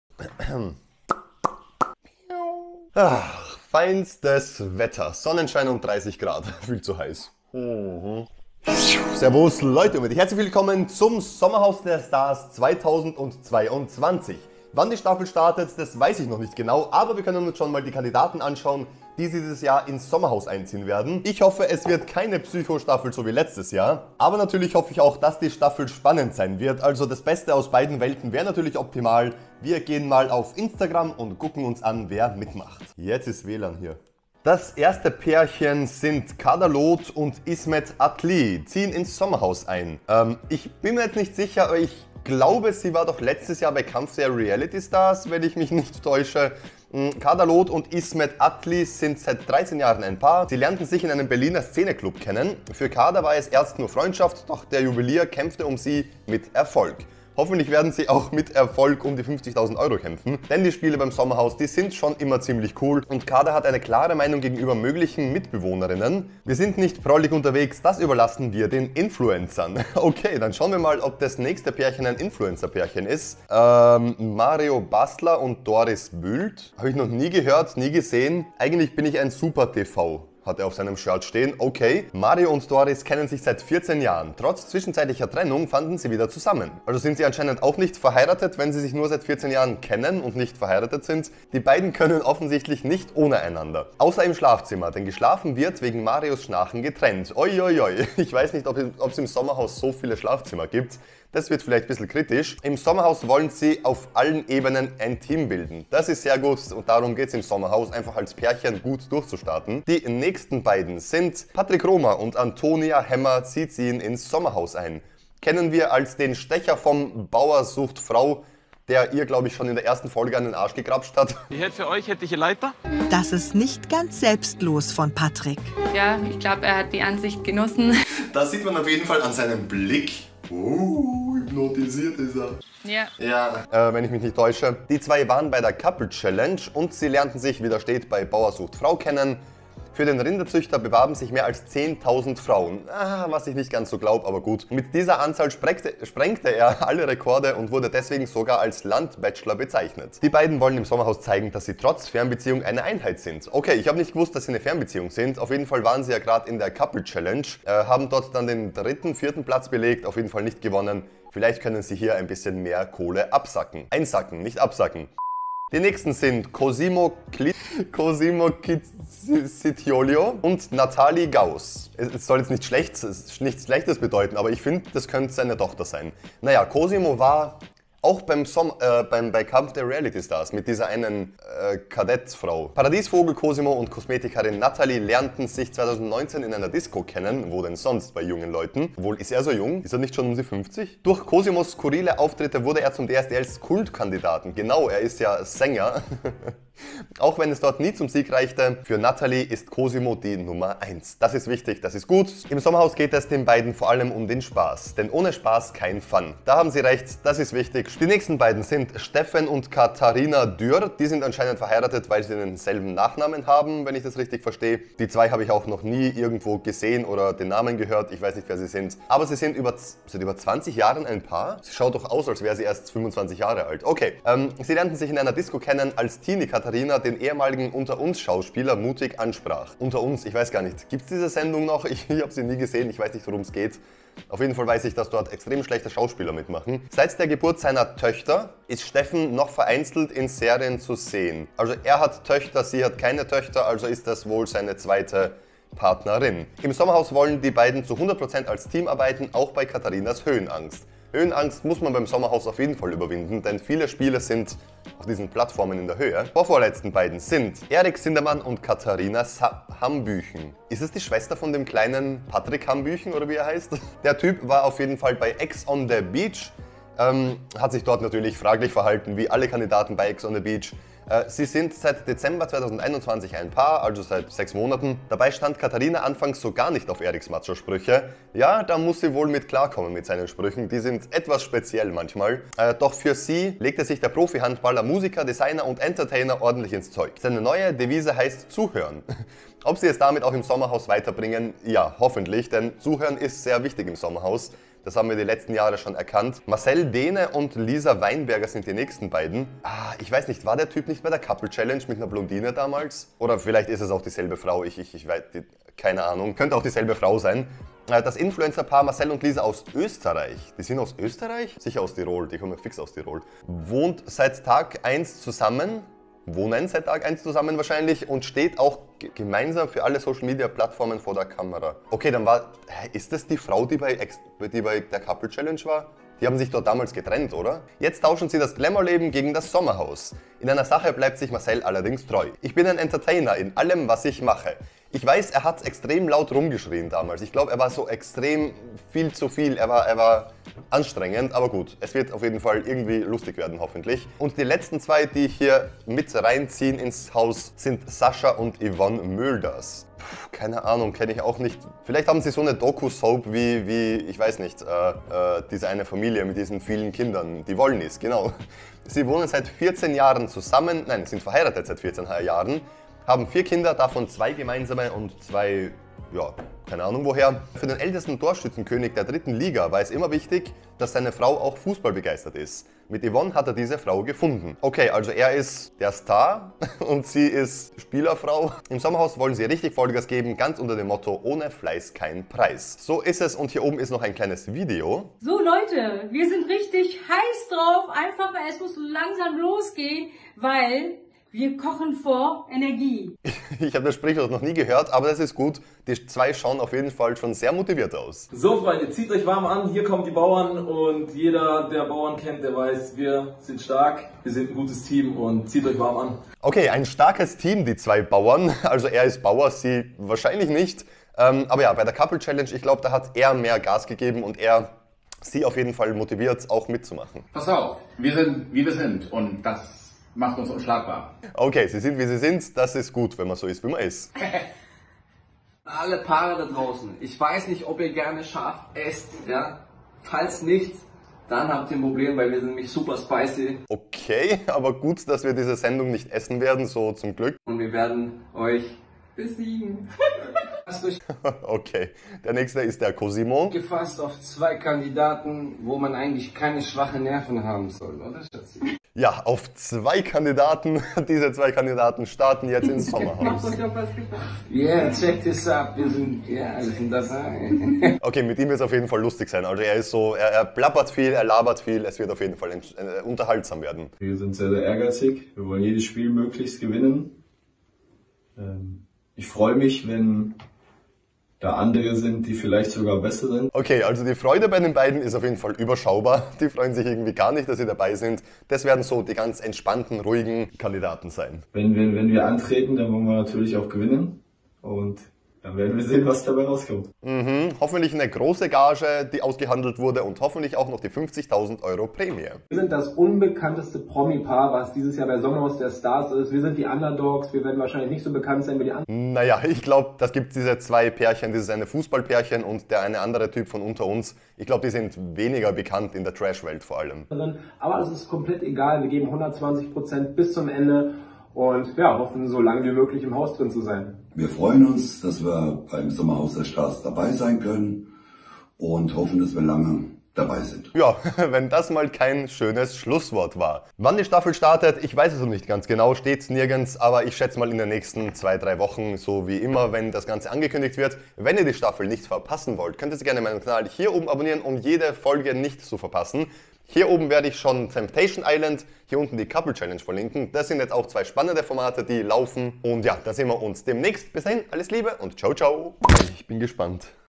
Weniger KRANK dieses Jahr?!? | Sommerhaus der Stars 2022 Die Kandidaten
Servus Leute,
das sommerhaus der stars staffel 7 ist ein komplettes Chaos. Promi-Paare in einer Bruchbude, Alkohol, Streithemen und ganz wenig Platz. Die Gewinner sind die die am längsten im Haus bleiben und bekommen 50.000€.

Das Sommerhaus der Stars startet bald und heute schauen wir uns gemeinsam an, wer ins Abrisshaus in Bochold einziehen darf. SommerhausderStars2022 Sommerhaus TVNOW Ich hoffe es gefällt euch!
Viel Spaß! Lasst gerne ein Abo und einen Daumen da! :-)